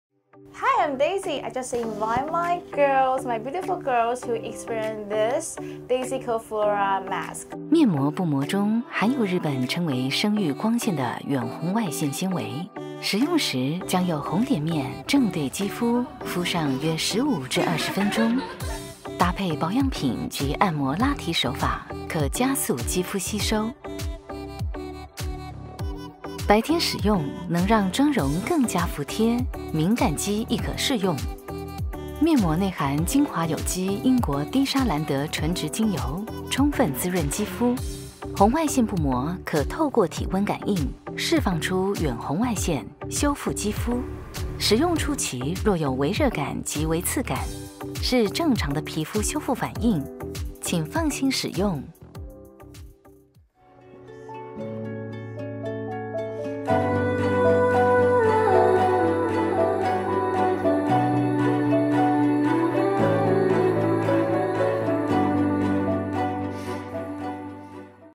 How to Make Your Skin Perfect With DaisyCodeFlora Day and Night /DaisyCode Flora 遠紅外線面膜使用步驟
Brighten and refresh your skin with Daisy! No matter day or night!
1: Put the side with red dots of the mask on your face for 15-20 minutes.
2: Apply lifting massage technique and/or combine your skincare products (EX: serum) to
    make quickly absorb.
3: Due to far-infrared technology, you may feel a little bit warm or tingling
    in the beginning.It’s a normal skin repaired process, and you can rest assured.
4. Also good for sensitive skin.

DaisyCode Flora 面膜，將日本稱之為生育光線的遠紅外線置於面膜布膜中，打開面膜將有紅點的那面對準肌膚貼上，敷完再將面膜從臉部輕輕撕開。睡前使用並配合現有晩霜按摩拉提，加速肌膚吸收，消除肌膚疲憊、改善膚質。

白天臉部清潔後使用面膜則可讓妝容更服貼，面膜內含極致珍稀純質有機英國滴莎蘭德(Tisserand) 純質精油，透過體溫感應釋放出遠紅外線，使用初期，會有微熱及微刺感，這都是使用遠紅外線面膜正常的皮膚修復反應，敏感肌也能安心使用。

DaisyCode

微信公眾號
微信搜尋 DaisyCode